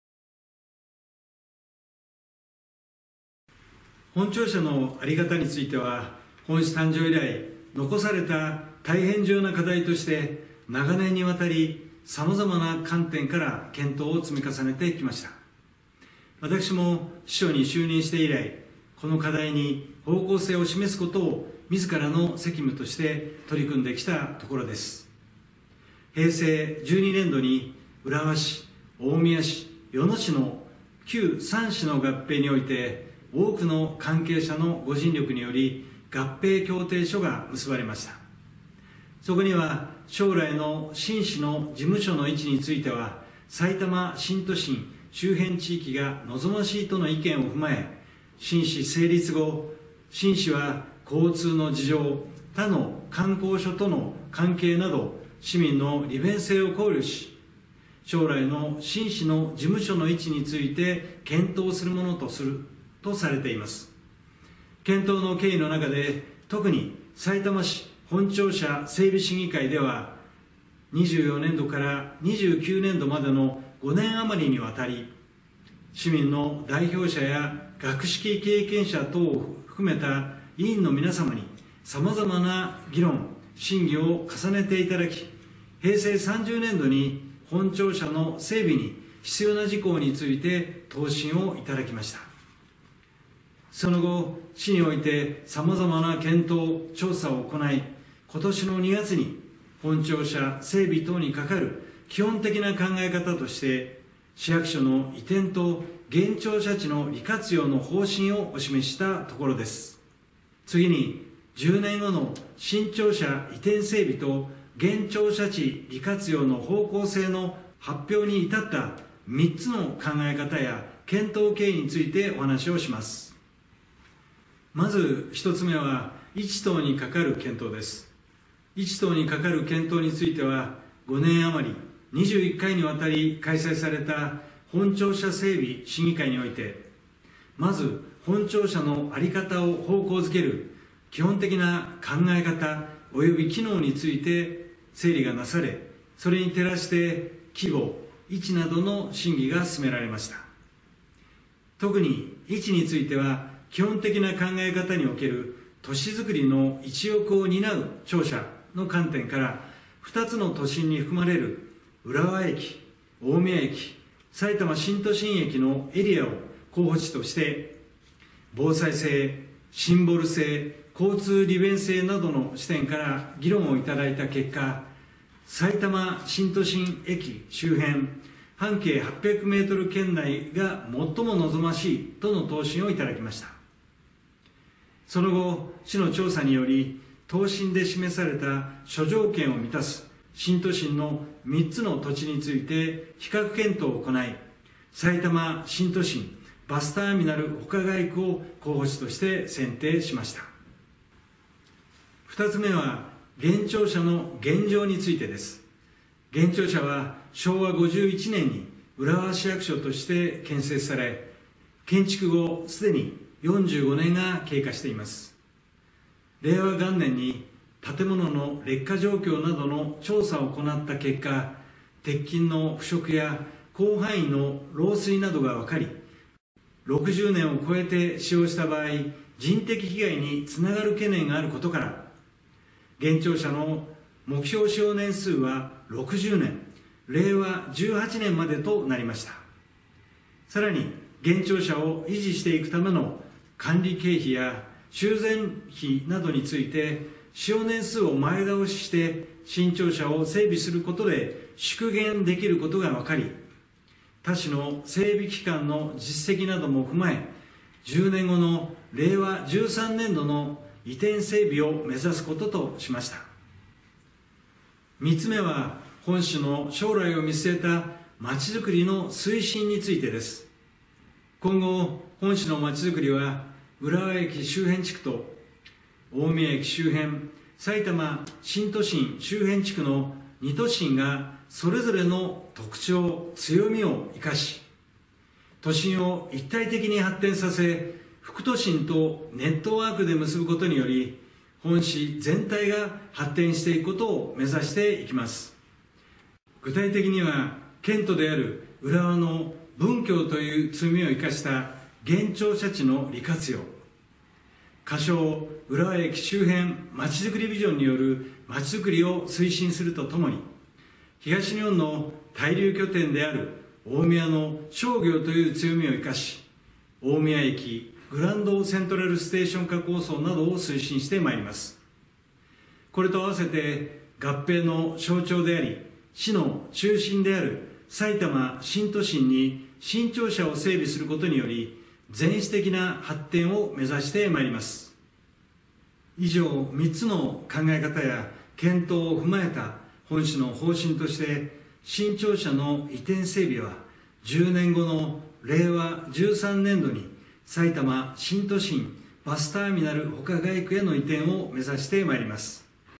分割版② 本庁舎整備等について(これまでの経緯、移転の必要性など)
さいたま市役所本庁舎は、10年後の令和13年度を目途にさいたま新都心に移転整備するとともに、現庁舎地は市民サービスの拠点である浦和区役所、浦和消防署の機能を残しつつ、新たな利活用を図ることとしています。
本動画は、(市長説明動画)分割版②「本庁舎整備等について(これまでの経緯、移転の必要性など)」になります。